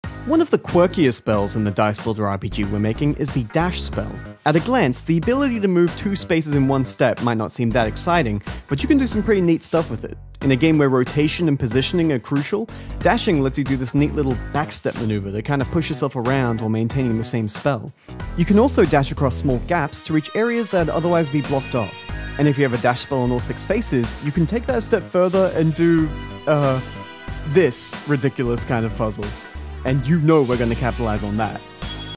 Spell spotlight: Dash!! 💨✨
We’re making a dice-builder RPG! It’s in pretty early stages right now but we have many many ideas to share. 🎲✨